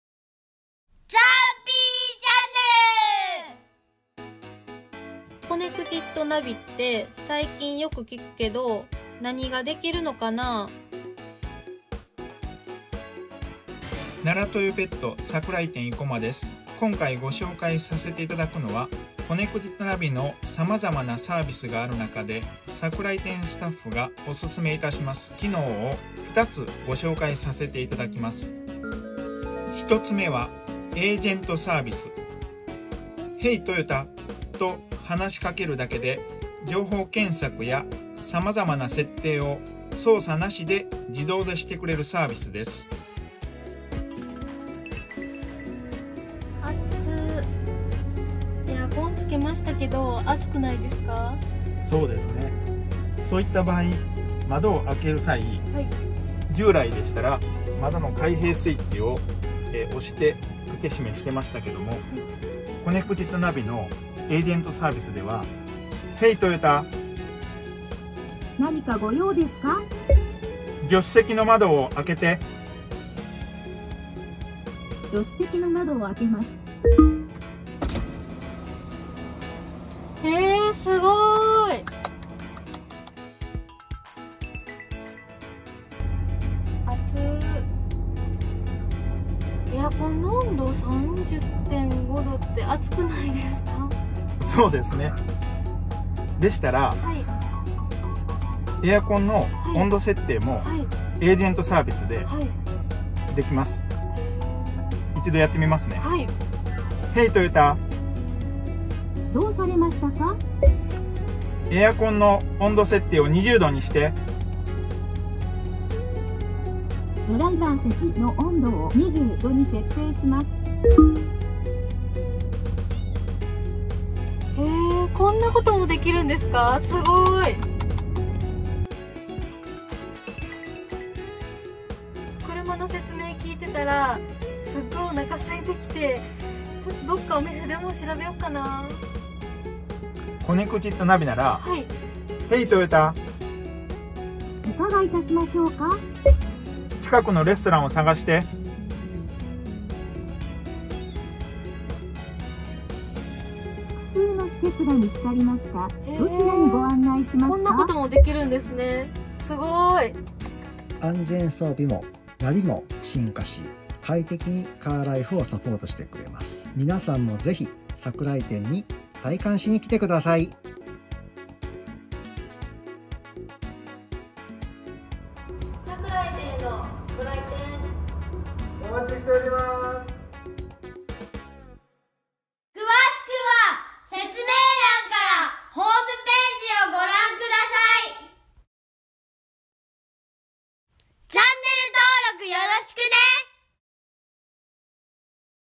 【VOL.237】コネクティッドナビって何ができるの？おすすめの便利機能をご紹介 奈良トヨペット桜井店
新型ノア・新型ヴォクシーから搭載の『コネクティッドナビ』って一体何ができるの？
そのように思われている方が多いのではないでしょうか？
今回は、そんな『コネクティッドナビ』の桜井店スタッフおすすめ便利機能をご紹介いたします♪
「ヘイ！トヨタ」あなたもコネクティッドナビに話しかけてみてくださいね！

▶チャプター
00:00 オープニング
00:28 おすすめ機能①　エージェント（音声対話サービス）
00:42 窓の開閉
01:22 エアコン操作
02:08 おすすめ機能②　コネクティッドナビ
02:40 エンディング

トヨタユナイテッド奈良（奈良トヨペット×ネッツトヨタ奈良）